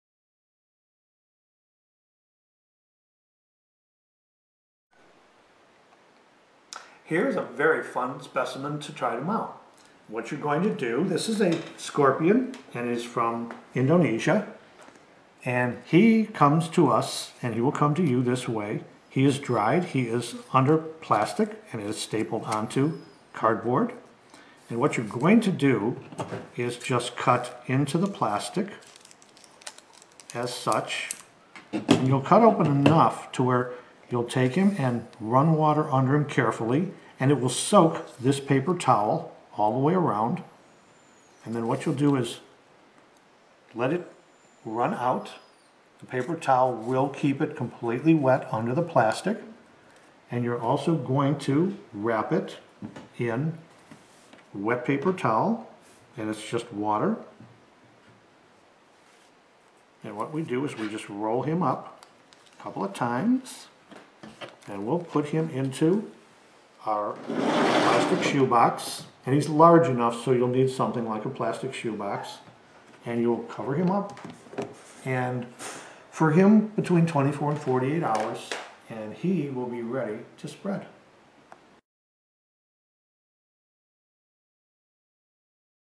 Rehydrating a Scorpion
How to Rehydrate/ or Relax a Scorpion for mounting